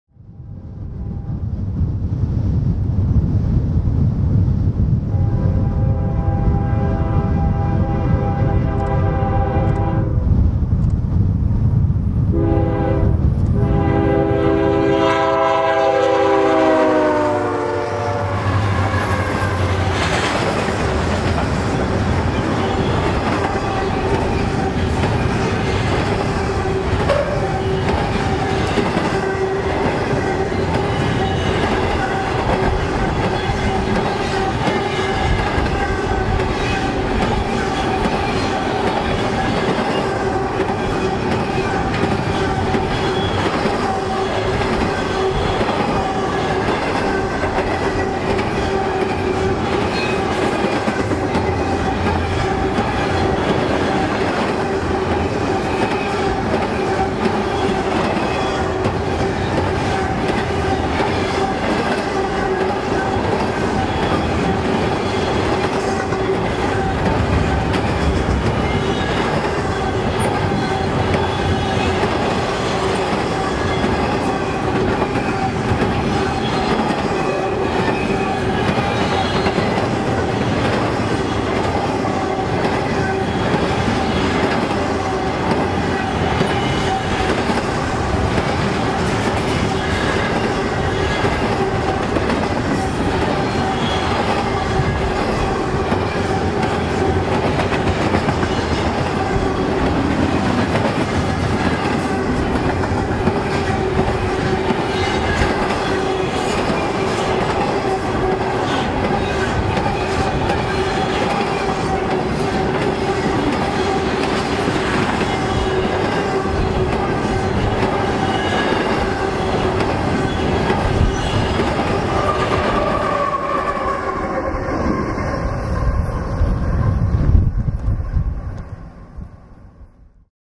UP7607 East - Orchard, ID
Rocking and rolling with clear signals ahead, the UP7607 with UP4929 and UP3929, as UP7718 does the rear DPU duties, clears Orchard on the mainline at 10:55.  The old water tower still remains, but mother nature continues to torture her as the years go by.  Orchard was the east junction of the old line into Boise, although these days it serves as a home for stored railcars quite often, longing for the day when something may roll along the rails again with regular occurrence.